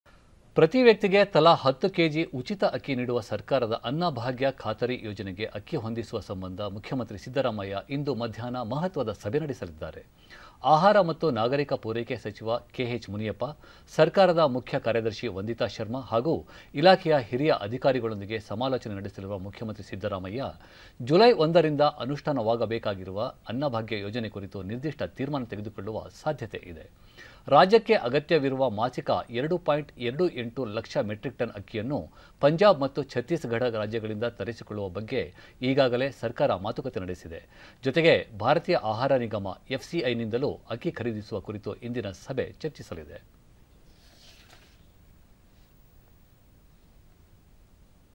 ಹಿರಿಯ ಅಧಿಕಾರಿಗಳೊಂದಿಗೆ ಇಂದು ಮುಖ್ಯಮಂತ್ರಿ ಸಭೆ.
ಪ್ರತಿ ವ್ಯಕ್ತಿಗೆ ತಲಾ 10 ಕೆ.ಜಿ. ಉಚಿತ ಅಕ್ಕಿ ನೀಡುವ  ಸರ್ಕಾರದ ಅನ್ನಭಾಗ್ಯ ಖಾತರಿ ಯೋಜನೆಗೆ ಅಕ್ಕಿ ಹೊಂದಿಸುವ ಸಂಬಂಧ ಮುಖ್ಯಮಂತ್ರಿ ಸಿದ್ದರಾಮಯ್ಯ ಇಂದು ಮಧ್ಯಾಹ್ನ ಮಹತ್ವದ ಸಭೆ ನಡೆಸಲಿದ್ದಾರೆ.
ಆಹಾರ ಮತ್ತು ನಾಗರಿಕ ಪೂರೈಕೆ ಸಚಿವ ಕೆ.ಎಚ್. ಮುನಿಯಪ್ಪ, ಸರ್ಕಾರದ ಮುಖ್ಯ ಕಾರ್ಯದರ್ಶಿ ವಂದಿತಾ ಶರ್ಮಾ ಹಾಗೂ ಇಲಾಖೆಯ ಹಿರಿಯ ಅಧಿಕಾರಿಗಳೊಂದಿಗೆ ಸಮಾಲೋಚನೆ ನಡೆಸಲಿರುವ ಮುಖ್ಯಮಂತ್ರಿ ಸಿದ್ದರಾಮಯ್ಯ, ಜುಲೈ 1ರಿಂದ ಅನುಷ್ಠಾನವಾಗಬೇಕಿರುವ ಅನ್ನಭಾಗ್ಯ ಯೋಜನೆ ಕುರಿತು ನಿರ್ದಿಷ್ಠ ತೀರ್ಮಾನ ತೆಗೆದುಕೊಳ್ಳುವ ಸಾಧ್ಯತೆಯಿದೆ.
ರಾಜ್ಯಕ್ಕೆ ಅಗತ್ಯವಿರುವ ಮಾಸಿಕ 2.28 ಲಕ್ಷ ಮೆಟ್ರಿಕ್ ಟನ್ ಅಕ್ಕಿಯನ್ನು ಪಂಜಾಬ್ ಮತ್ತು ಛತ್ತೀಸ್ ಗಢ ರಾಜ್ಯಗಳಿಂದ ತರಿಸಿಕೊಳ್ಳುವ ಬಗ್ಗೆ ಈಗಾಗಲೇ ಸರ್ಕಾರ ಮಾತುಕತೆ ನಡೆಸಿದೆ. ಜೊತೆಗೆ ಭಾರತೀಯ ಆಹಾರ ನಿಗಮ - FCI ನಿಂದಲೂ ಅಕ್ಕಿ ಖರೀದಿಸುವ ಕುರಿತು ಇಂದಿನ ಸಭೆ ಚರ್ಚಿಸಲಿದೆ.